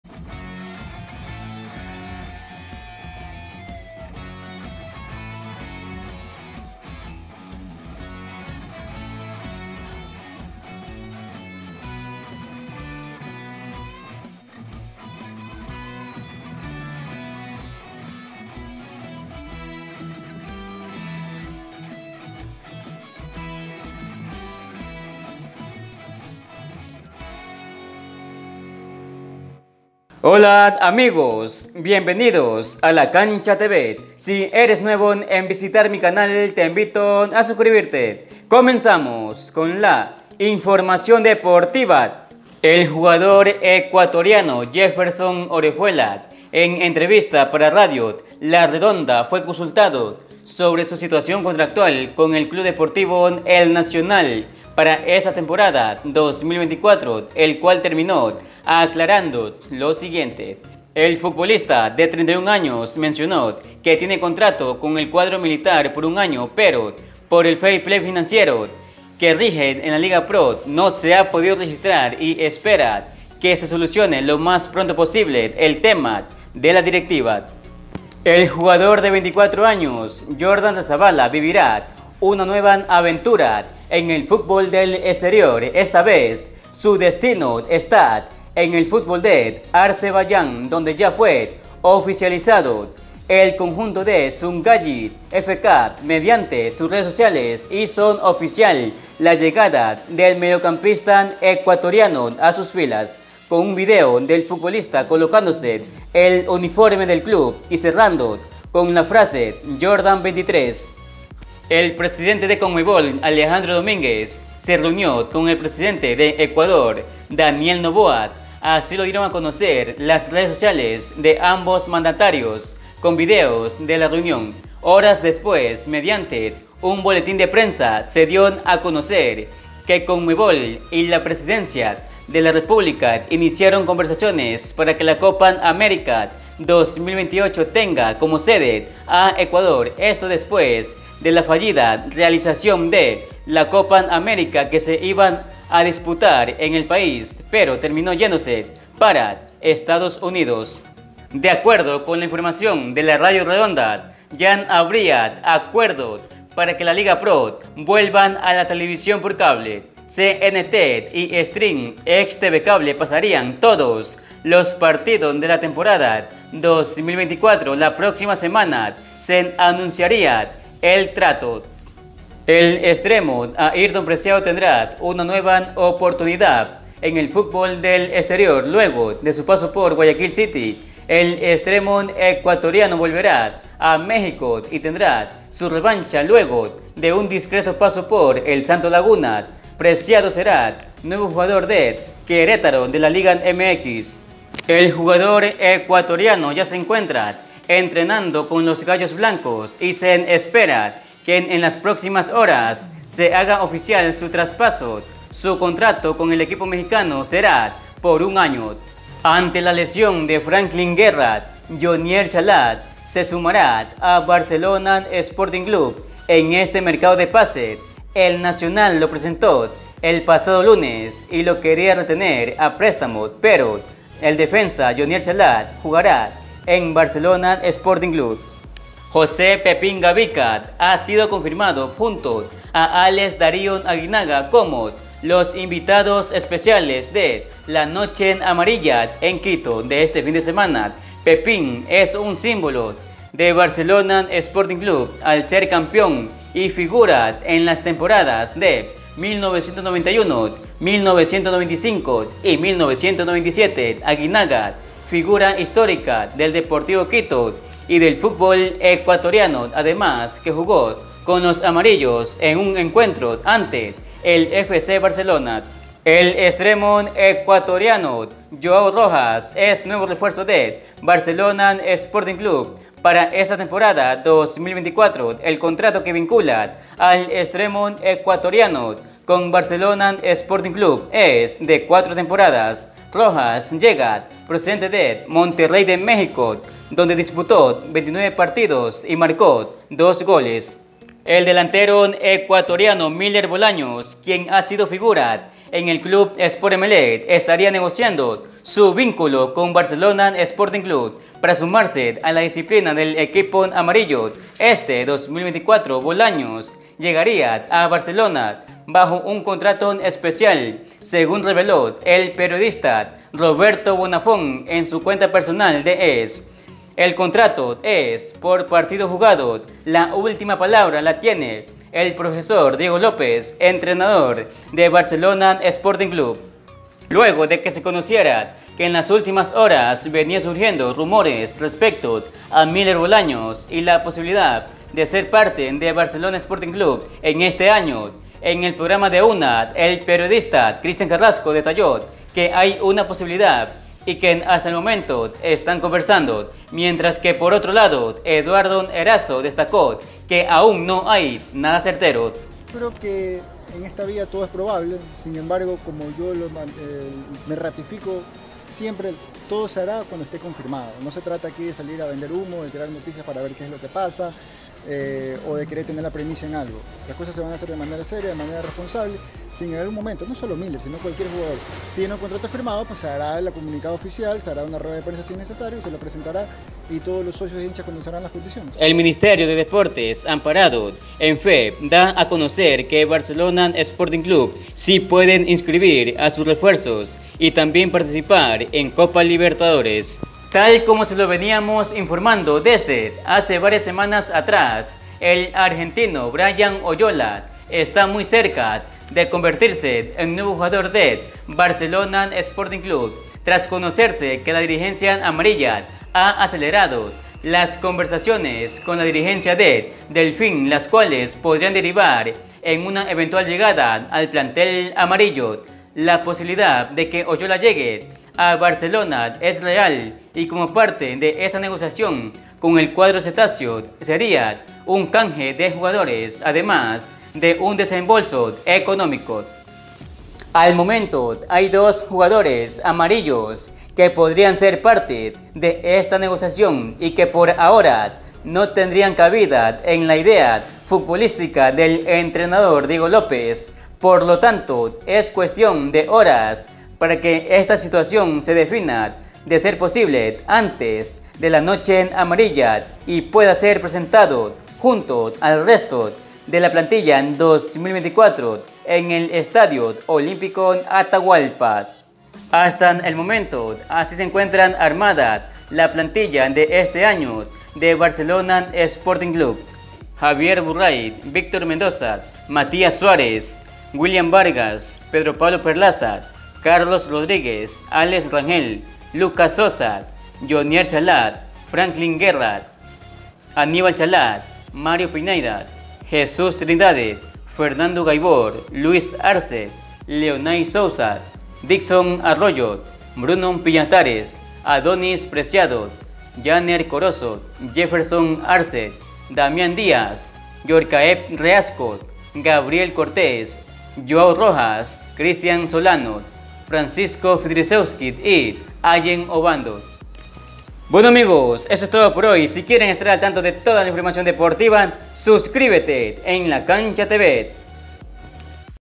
BRIAN OYOLA,MILLER BOLAÑOS Y JHONIER CHALÁ JUGARAN EN BSC|INVITADOS ESPECIALESPARA LA NOCHE AMARILLA
BRIAN OYOLA,MILLER BOLAÑOS Y JHONIER CHALÁ JUGARAN EN BSC|INVITADOS ESPECIALES PARA LA NOCHE AMARILLA|CONFIRMADO JOAO ROJAS JUGARA EN BSC Y MAS NOVEDADES DE BSC,JORDAN REZABALA AL FUTBOL DE ARZEBAIYAN,AYRTON PRECIADO A MEXICO,JEFFERSON OREJUELA SI JUGARA EN EL NACIONAL ,LIGAPRO SERA TRANSMITIDA POR TV CABLE,COPA AMERICA 2028 EN ECUADOR Y MAS NOVEDADES si quieren estar al tanto de toda la INFORMACIÓN DEPORTIVA SUSCRIBETE EN LA CANCHA TV